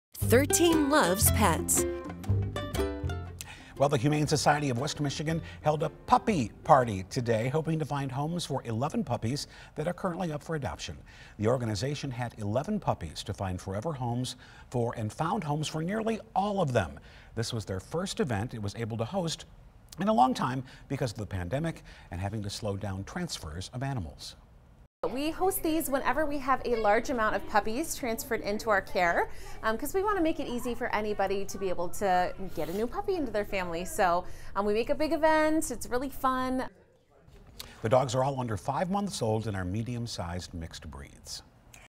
Humane Society hosts 'puppy party' to help puppies find forever homes
The Humane Society of West Michigan held a puppy party Friday, hoping to find homes for 11 puppies that are currently up for adoption.

This was the first event the organization was able to host in a long time due to the pandemic and having to slow down transfers of animals.

“We host these whenever we have a large amount of puppies transferred into our care, because we want to make it easy for anybody to be able to get a new puppy into their family,” said Marketing and Communications Coordinator Brianna Shahly. “So we make a big event. It's really fun.”